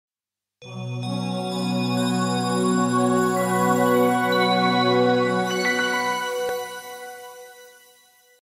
Logó animáció 1
Webdesing-Z ügyfelek számára készített rendelhető logó animáció. Az animációhoz saját logó szükséges átlátszó háttérrel jó minőségben.